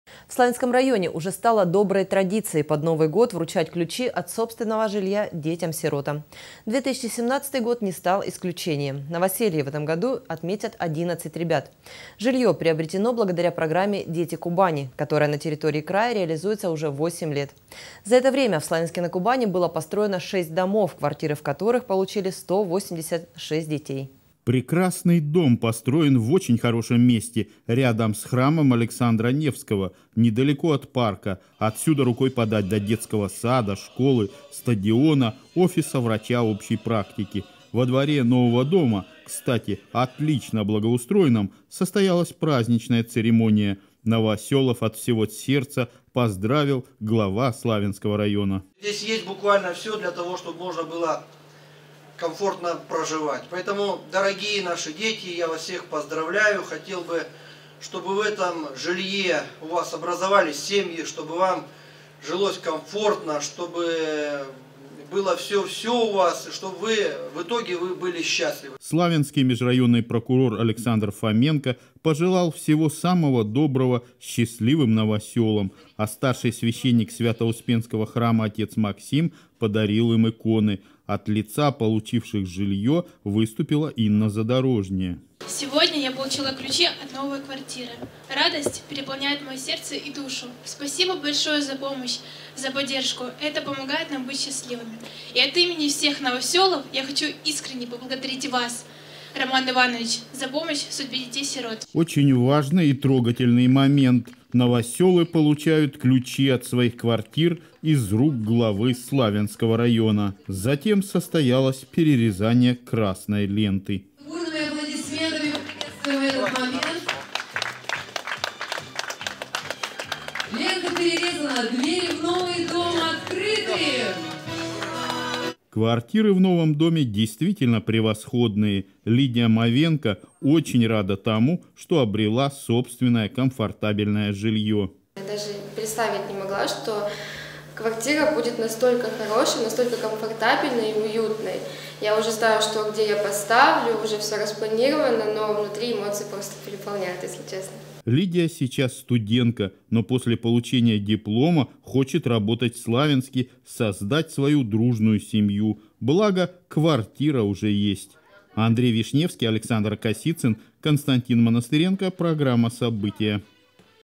Детям сиротам вручили ключи от собственного жилья
В Славянском районе уже стало доброй традицией  под новый год вручать ключи от собственного жилья детям-сиротам. 2017-й не стал исключением. Новоселье в этом году отметят 11 ребят. Жилье приобретено благодаря программе «Дети Кубани», которая на территории края реализуется уже 8 лет. За это время в Славянске-на-Кубани было построено 6 домов, квартиры в которых получили 186 детей.